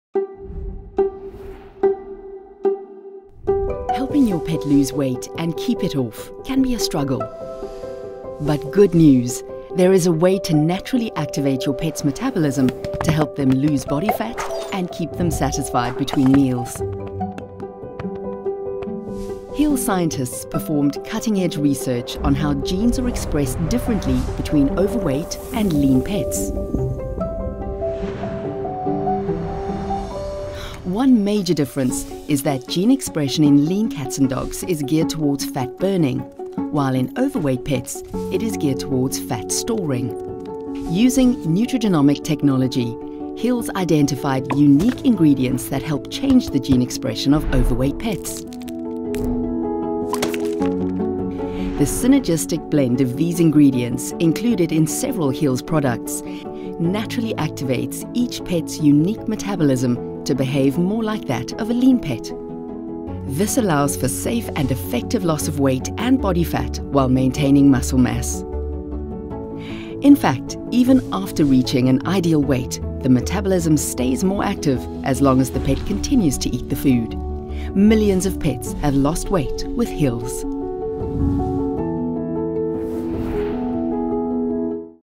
Hill's Pet Nutrition - Nutrigenomic technology
Hill's uses Nutrigenomic technology, where we select specific ingredients to alter the gene expression of overweight animals.